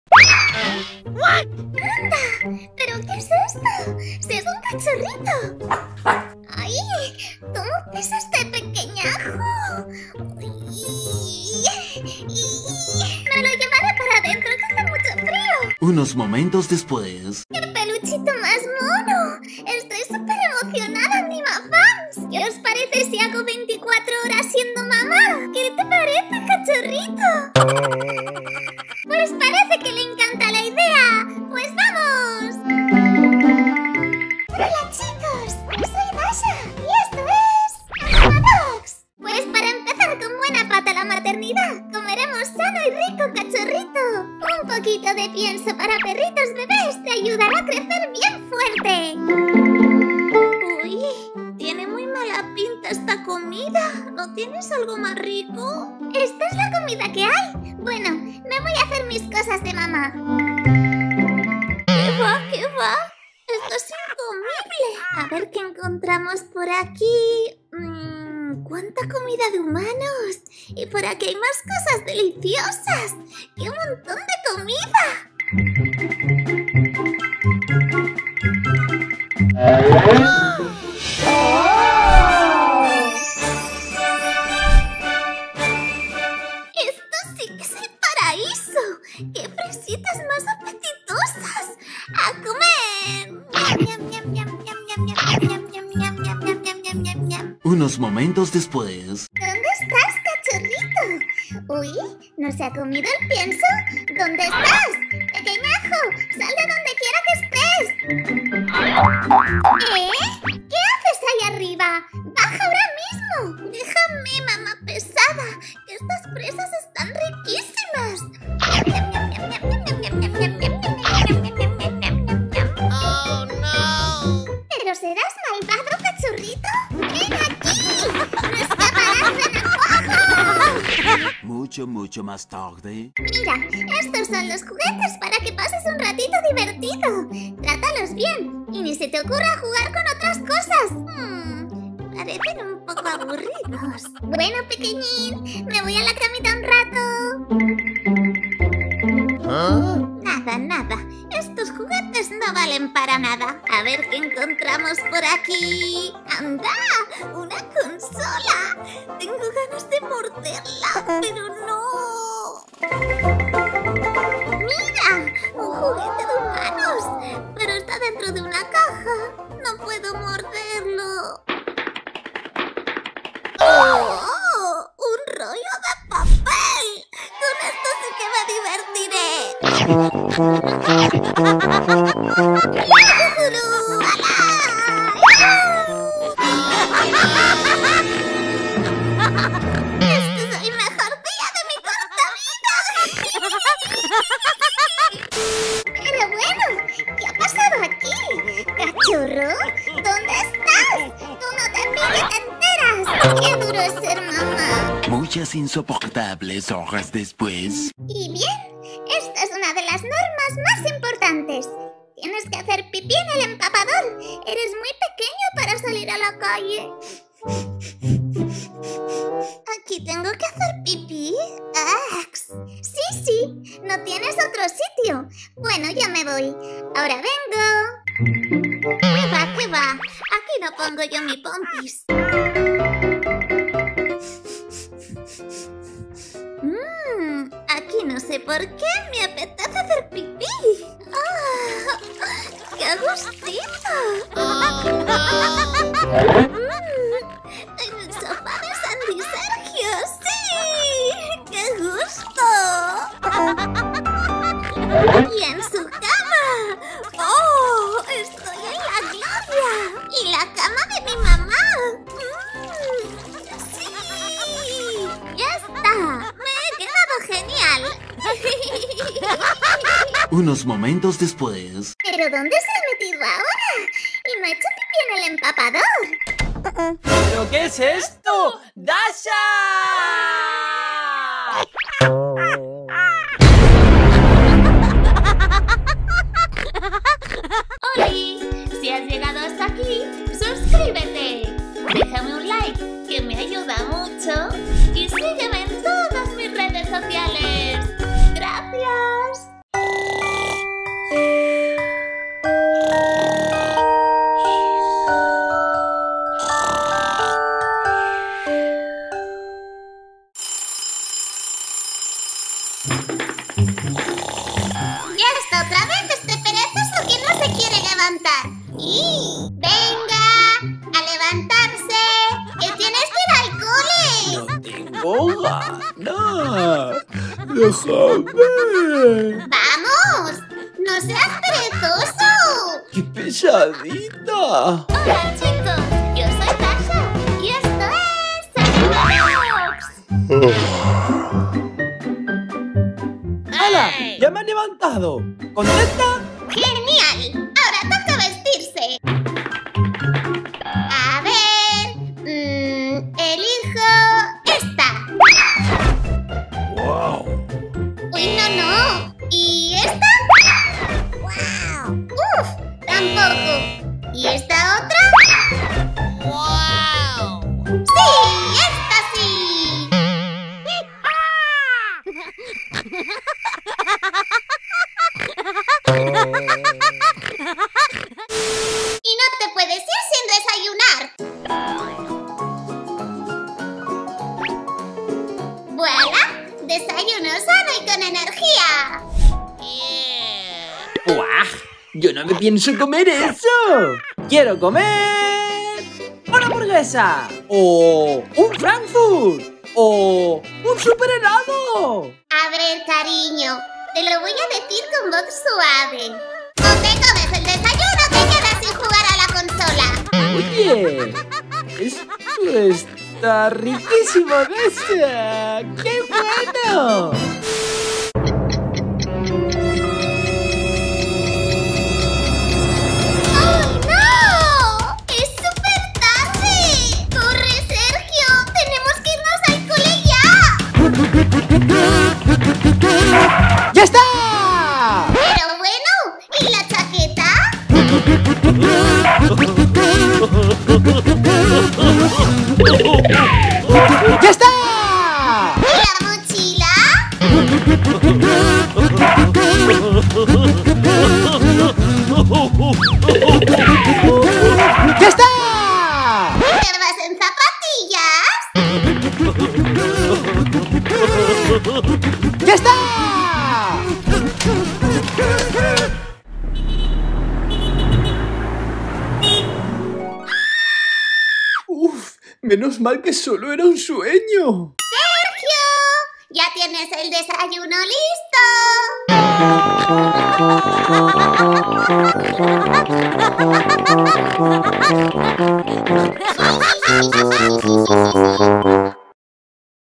A mi PERRO le gusta ser MAMÁ! 👶🏻🐶 Anima Dogs
A Dasha le gusta mucho hacer de mamá y en estos vídeos verás lo bien o lo mal que se le da. No te lo pierdas porque son super divertidos!